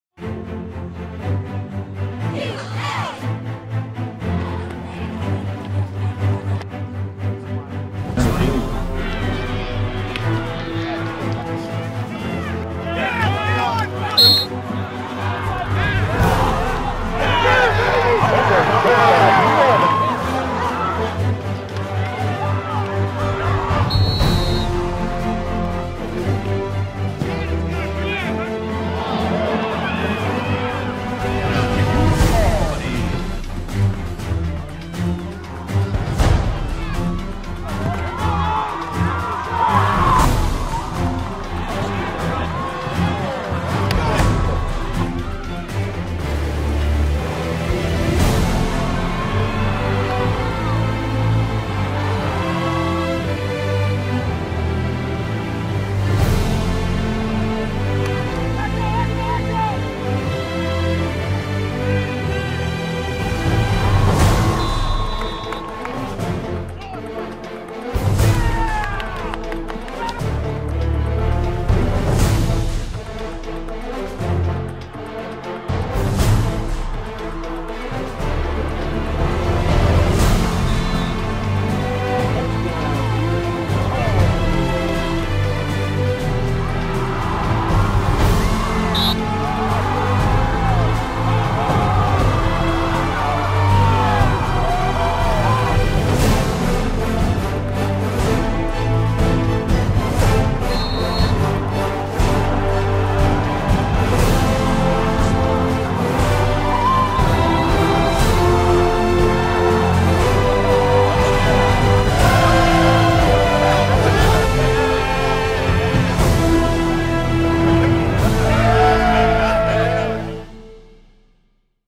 UAlbany Football Team Wins Sixth NEC Champion Title!
In the last home footbal game ever on University Field, UAlbany, led by legendary head coach Bob Ford, defeated Central Connecticut State 63-34 to capture a share of the NEC  Championship.